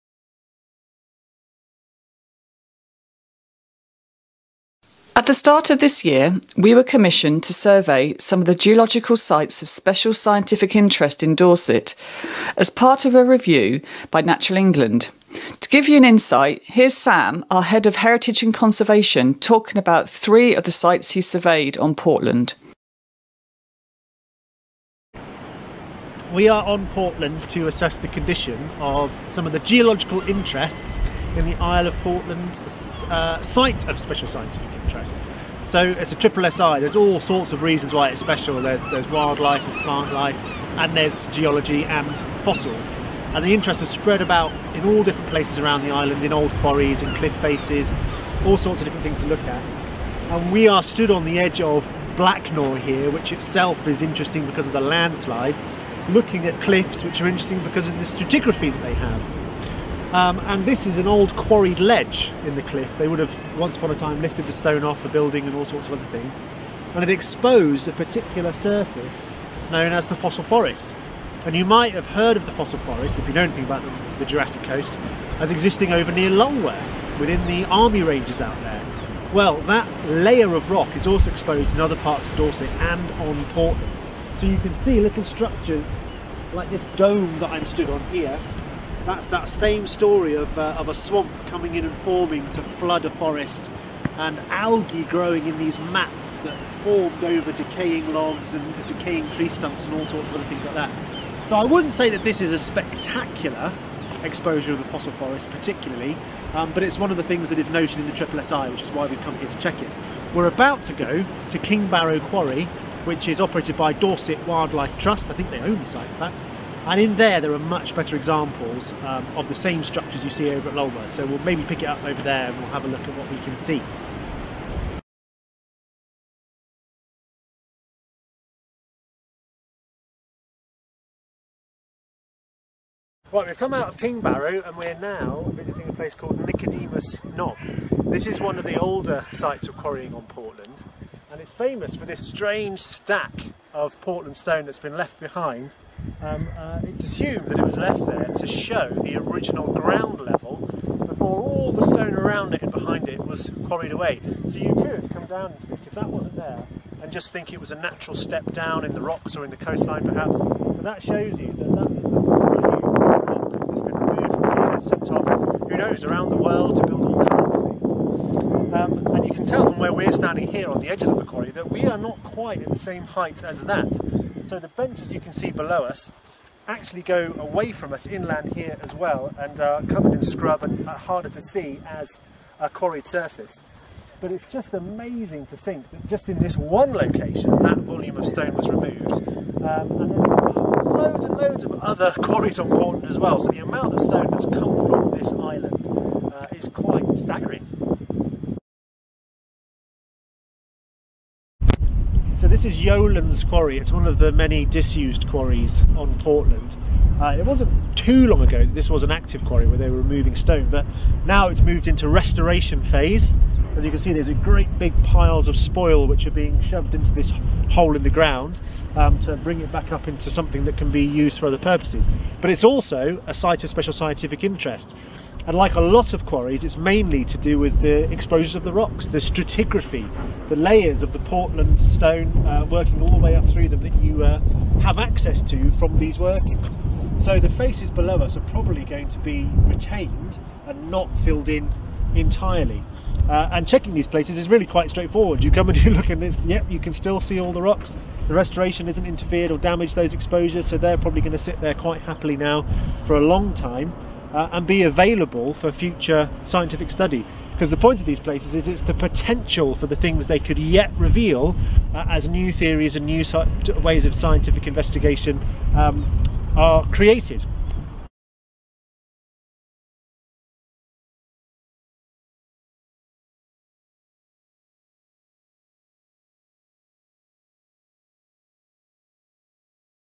Geological Survey of Portland
Sam Scriven, Head of Heritage and Conservation, describes our work in conjunction with Natural England surveying the geological Sites of Special Scientific Interest on the Isle of Portland.